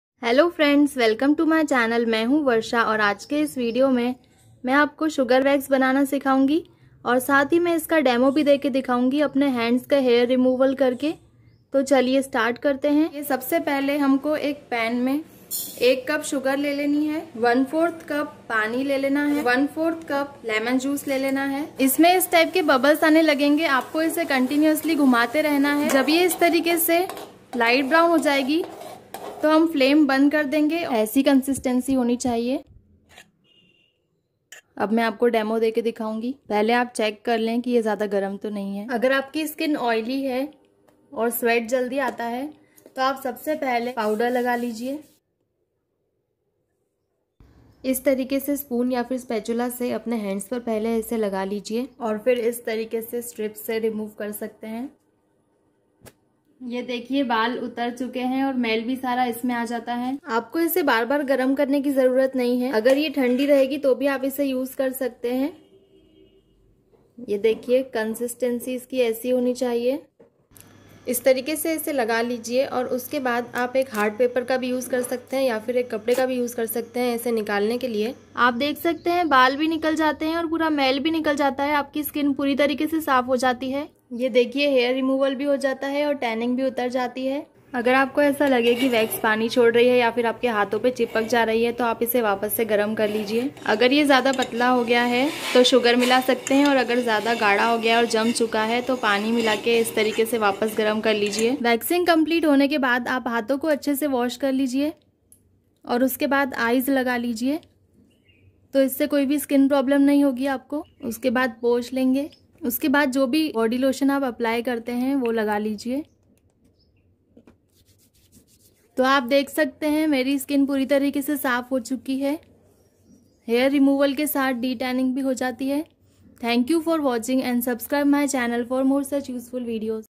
How to make sugar wax at home|Live Demonstration|DIY Sugar Wax Recipe and Tutorial|Sugar Lemon Water
Try this wax & u will see best results.